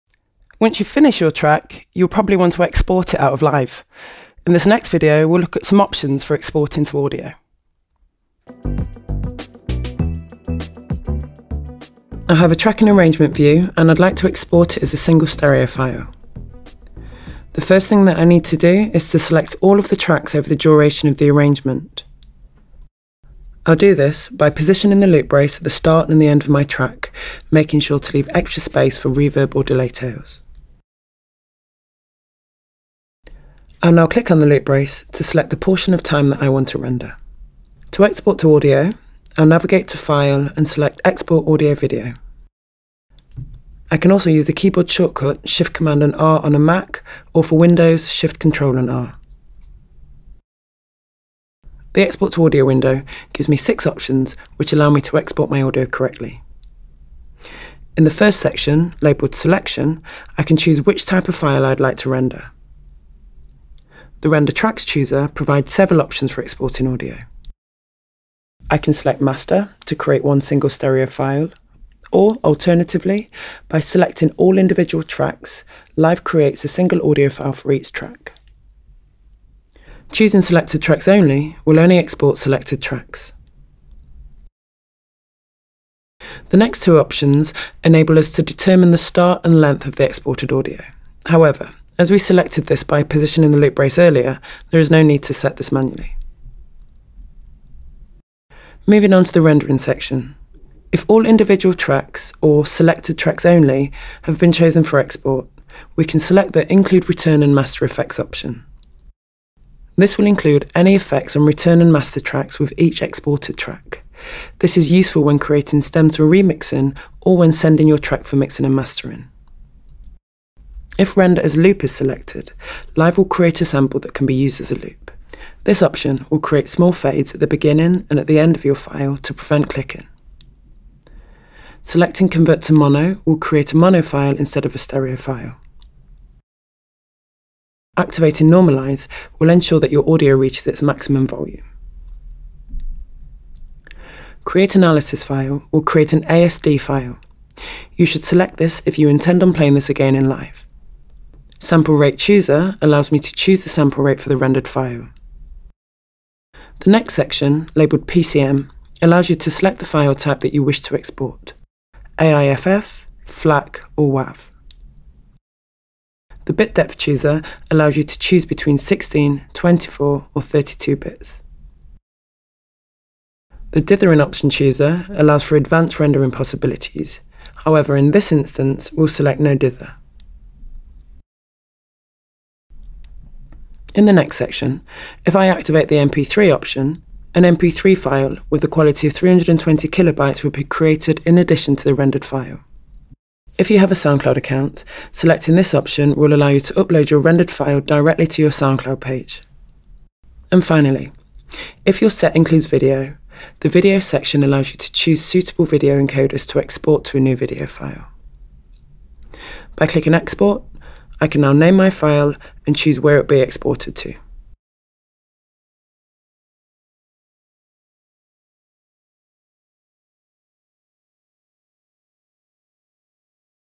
Learn Live: Exporting
Learn about the options for exporting audio in Ableton Live.